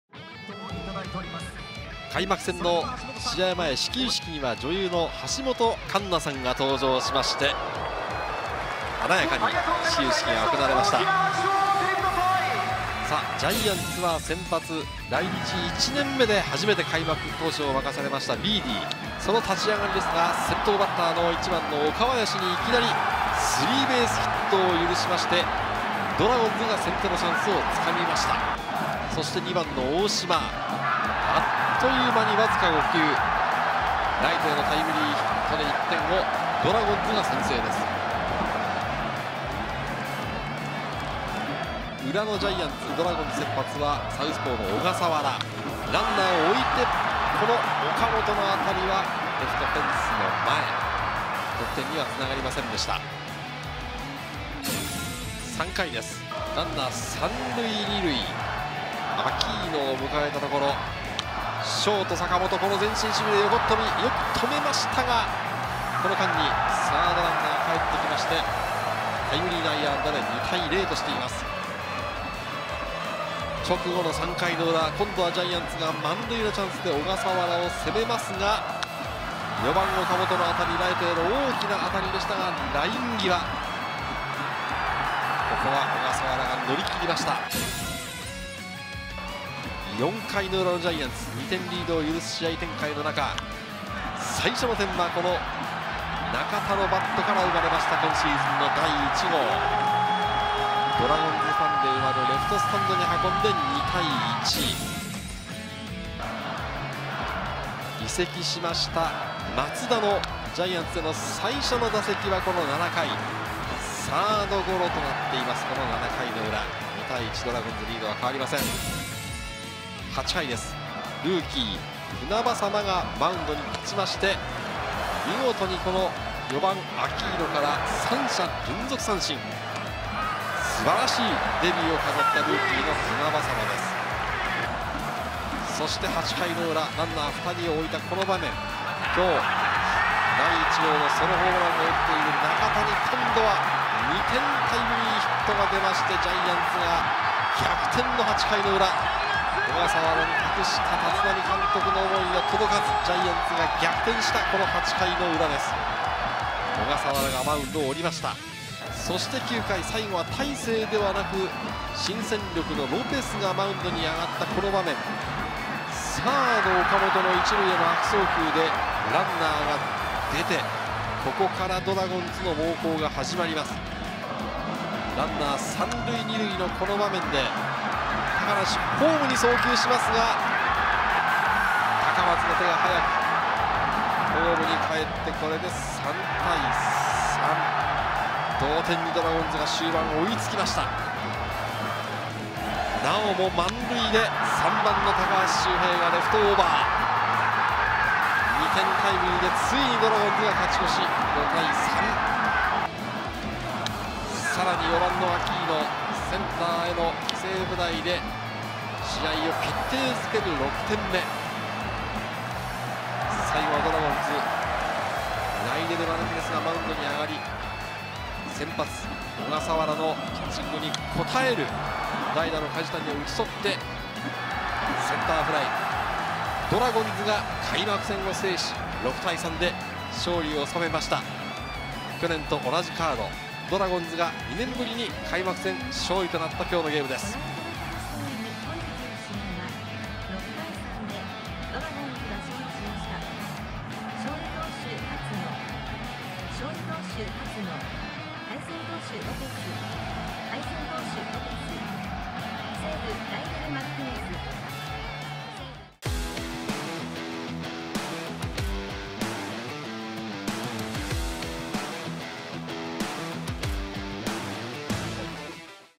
【ハイライト・3/31】開幕戦中日9回逆転勝利！高橋周勝ち越しタイムリー大島猛打賞！小笠原8回途中145球熱投【巨人×中日】【開幕戦】
2023年3月31日(金)
東京ドーム